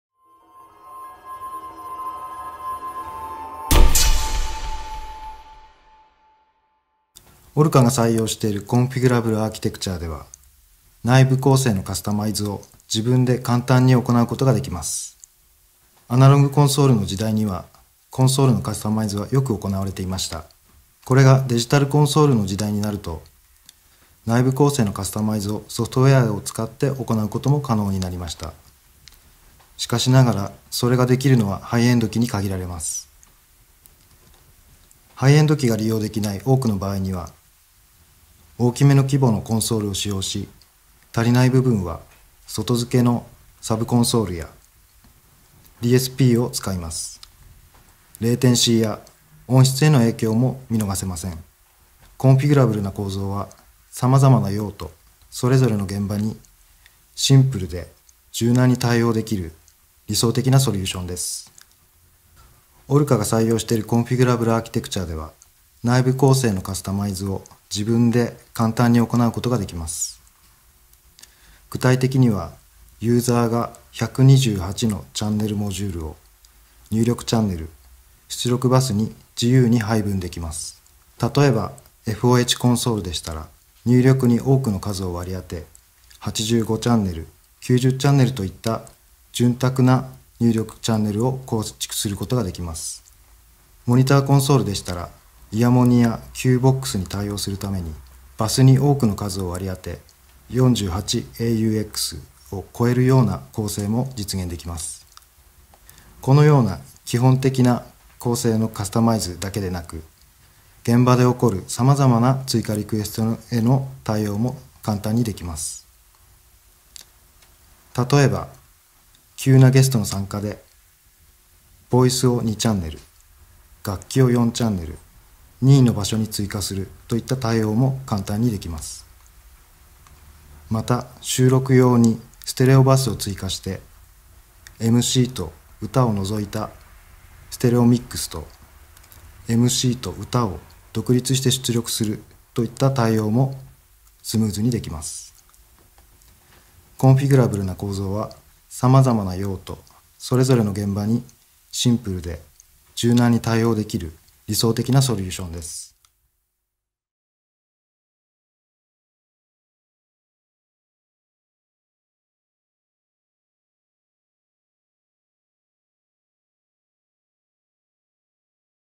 「O.H.R.C.A」 開発者インタビュー Vol.2 コンフィギュラブル構造
開発者が自ら語る「O.H.R.C.A」Live Mixing Console M-5000の特長。